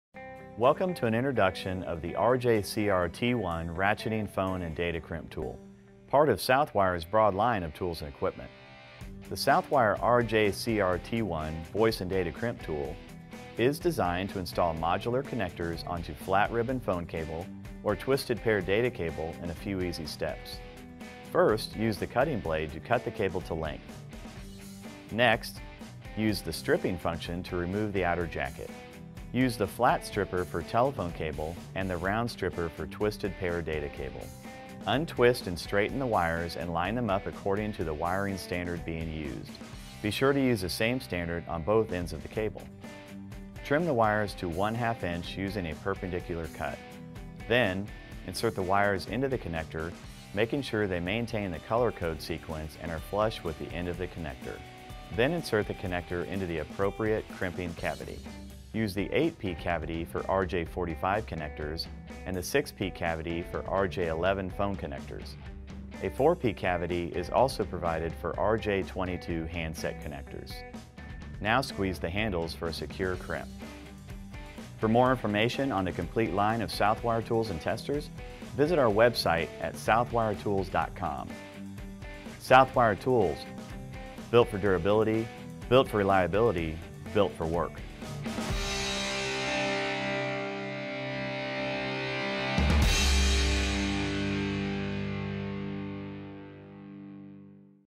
Ratcheting Phone & Data Crimp Tool
"The RJCR-T1 is a professional ratcheting Phone & Data Crimp Tool that cuts and strips voice and data cable, and crimps RJ45 (data) plus RJ11/RJ12 and RJ22 (voice) connectors.